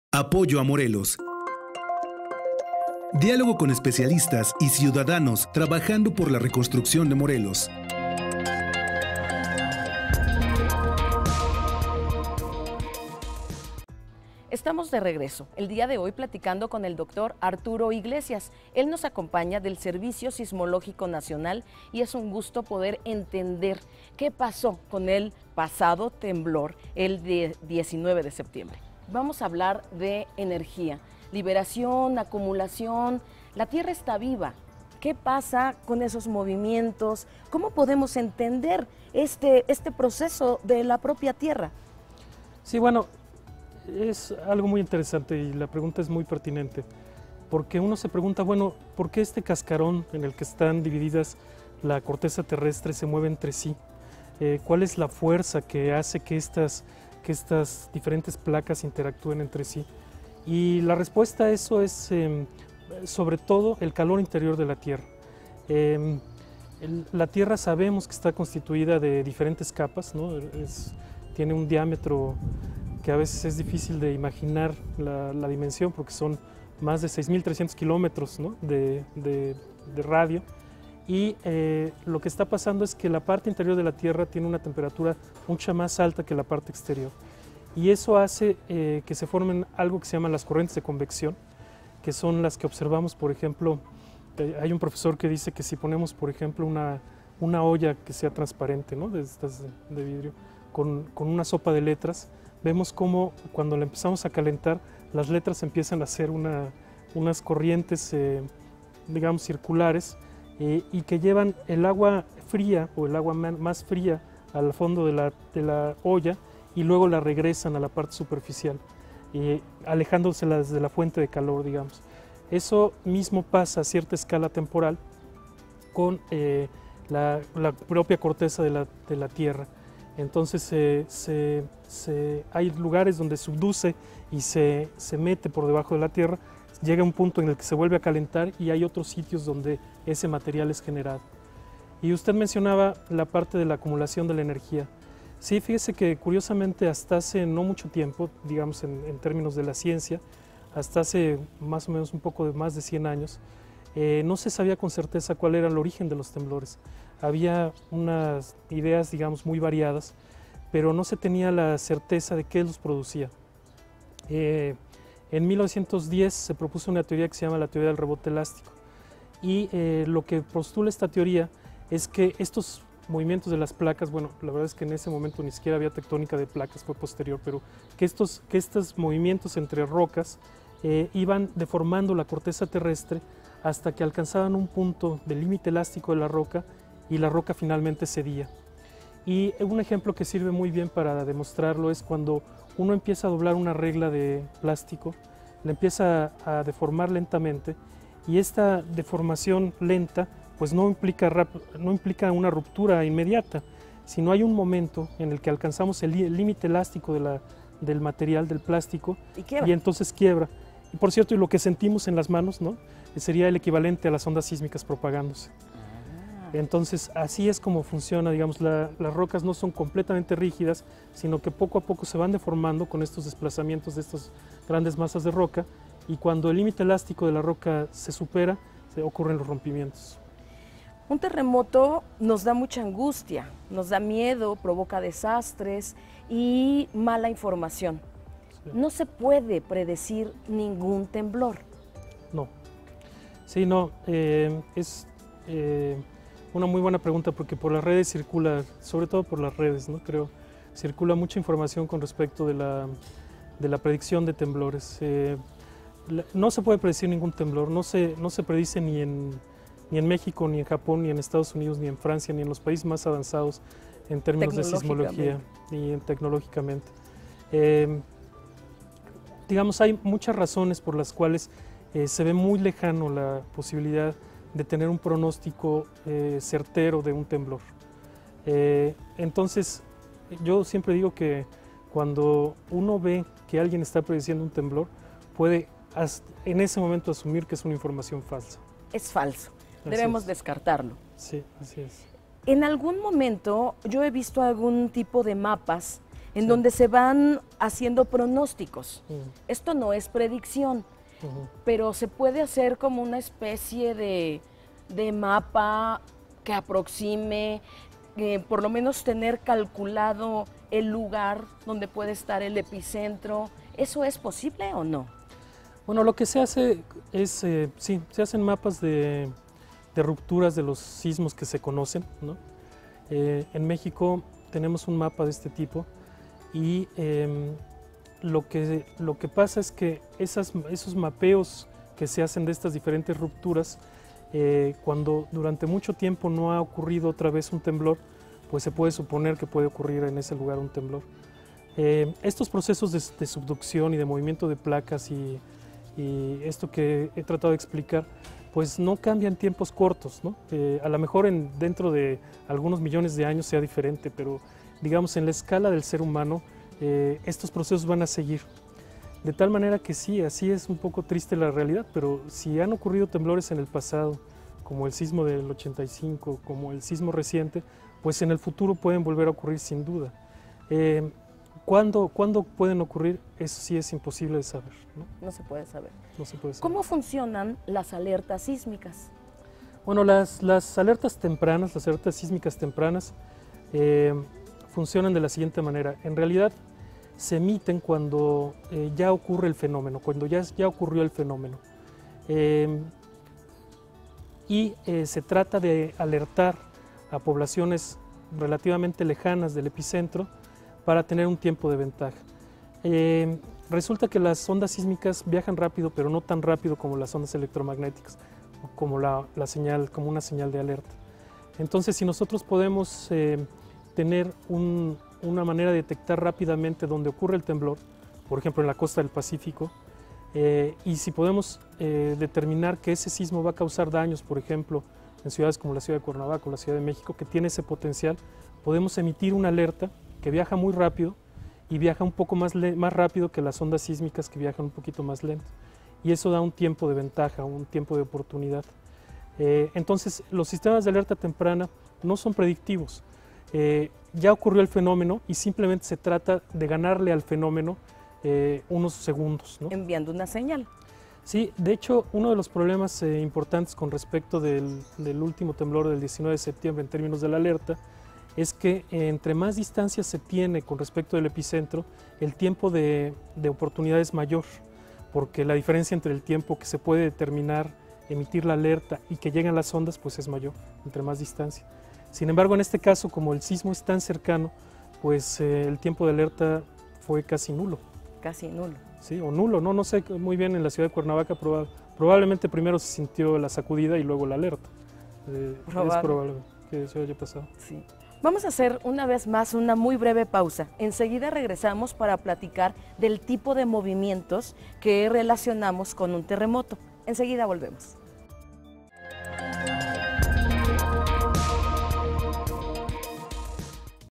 El sismo del 19 de Septiembre - parte 2
Dr. Arturo Iglesias
Departamento Sismológico del Instituto de Geofísica de la UNAM